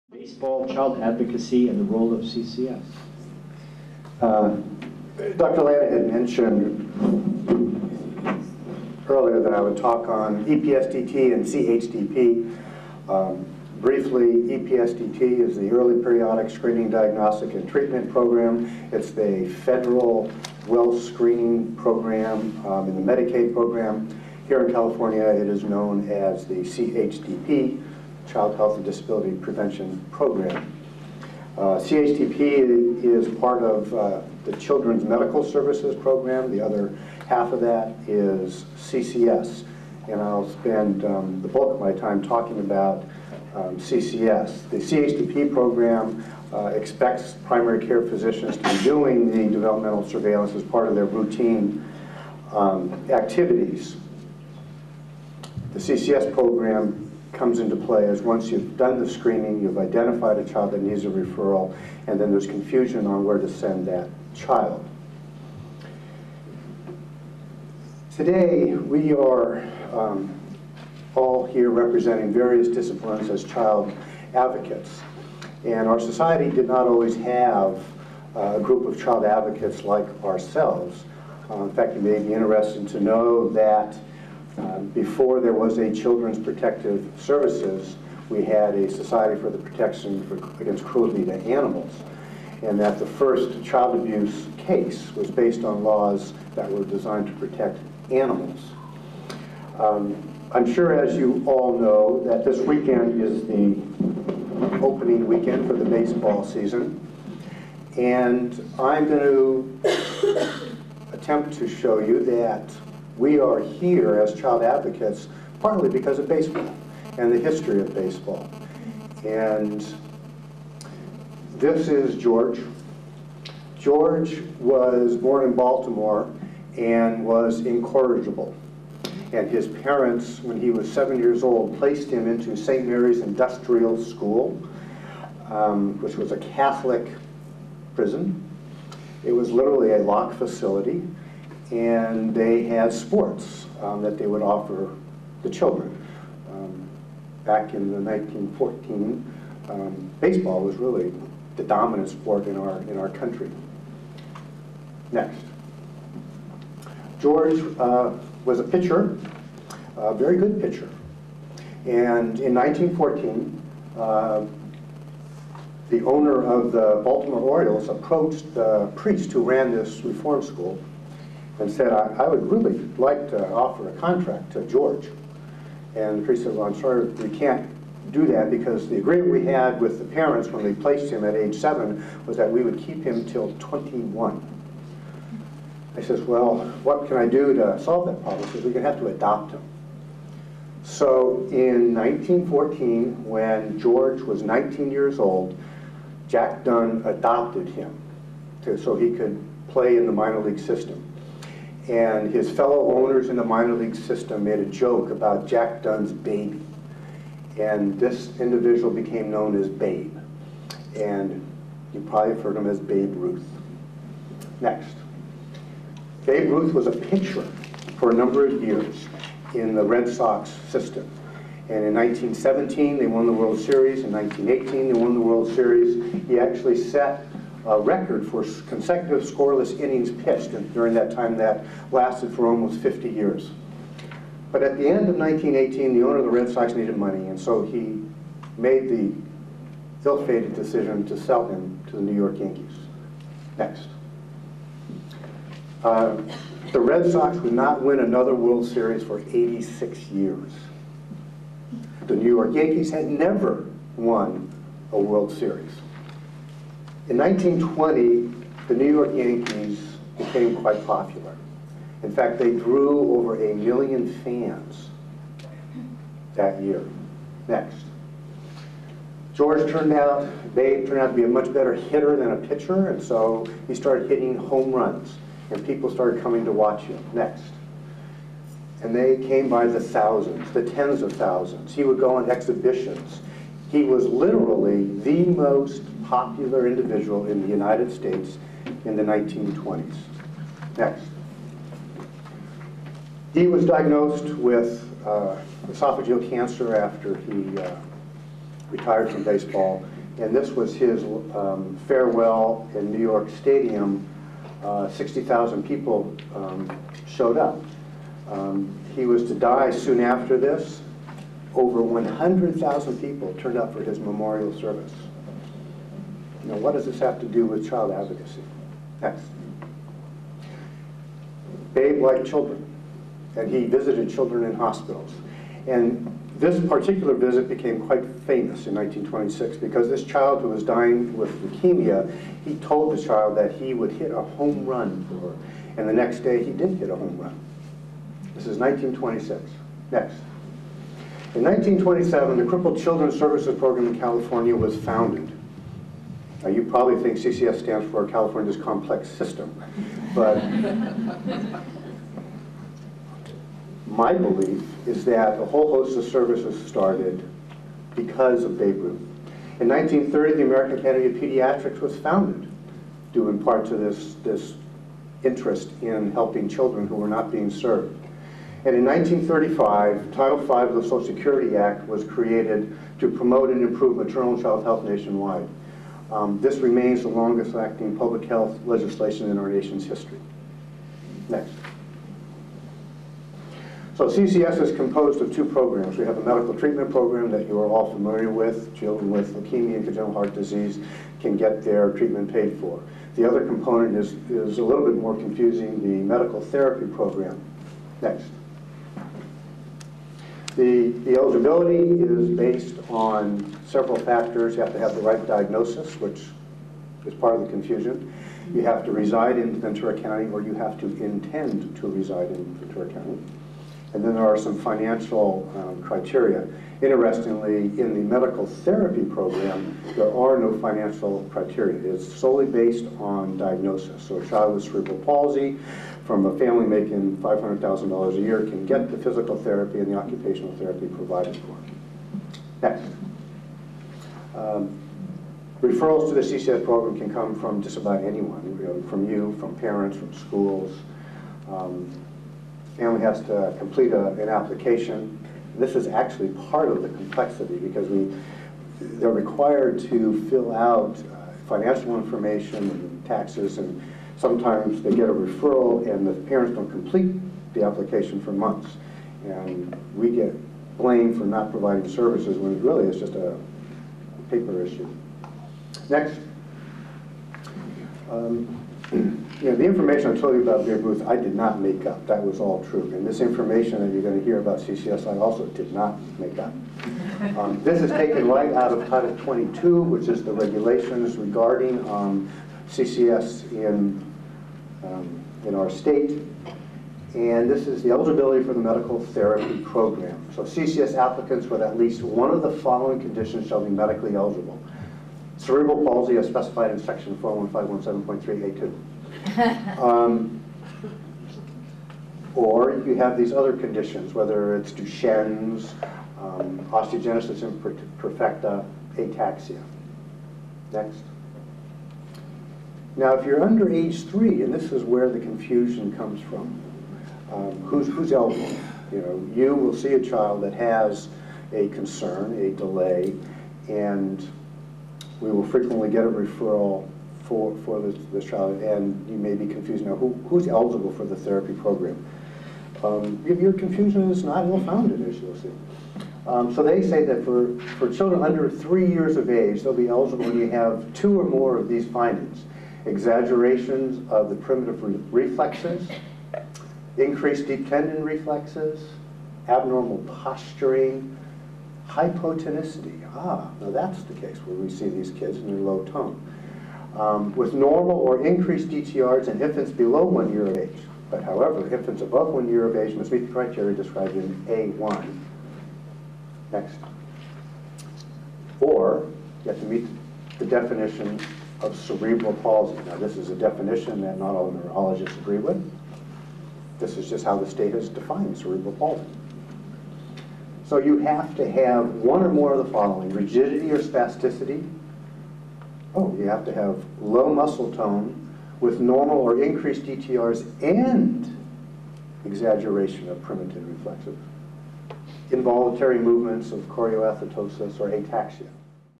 Paul Russell 1 - Developmental Screening Conference 2009
Paul Russell, 2009 Baseball, Child Advocacy and the Role of CCS at the Developmental Screening Conference 2009